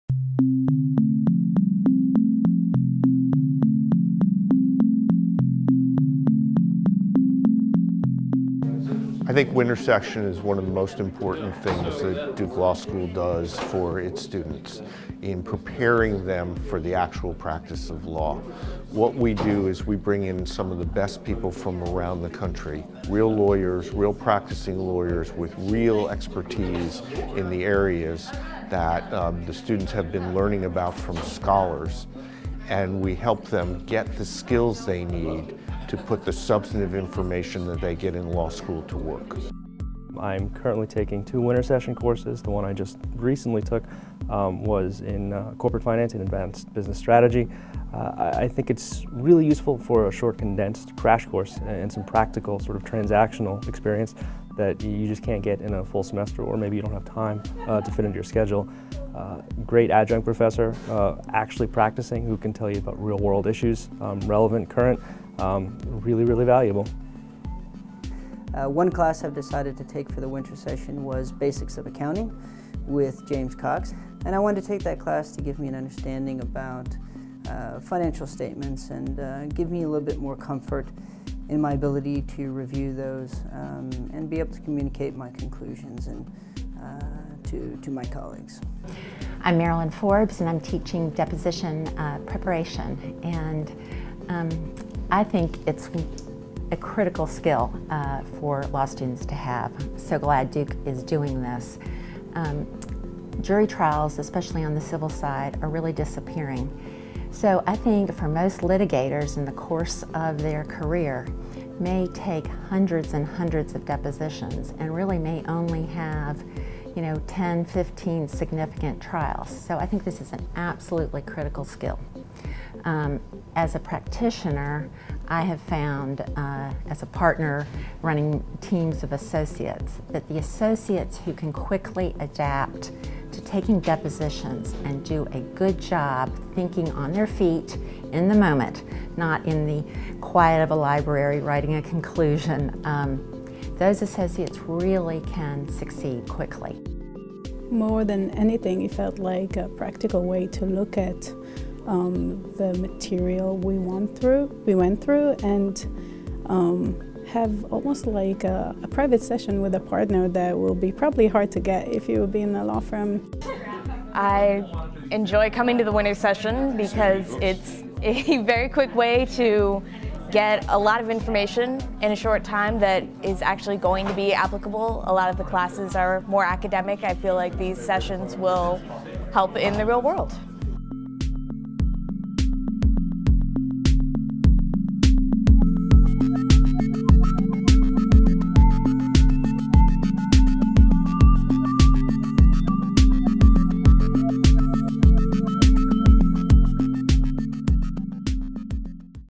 Duke Law 2012 Wintersession
During Wintersession Duke Law students have the opportunity to take a number of practical, professional skills development courses with leading practitioners and Duke Law faculty.